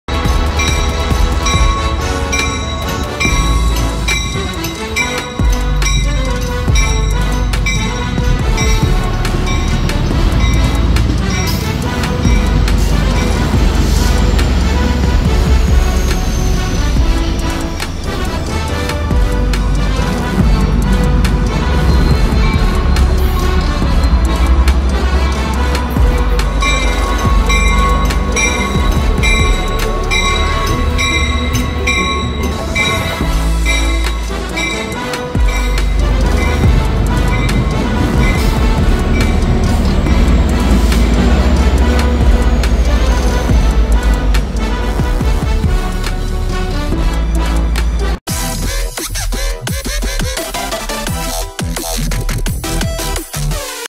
Texas Trinity Railway Express 570 Leaving Station! (Two Angles!!)
UNCUT FOOTAGE -  @SteamTrainsBoyExtras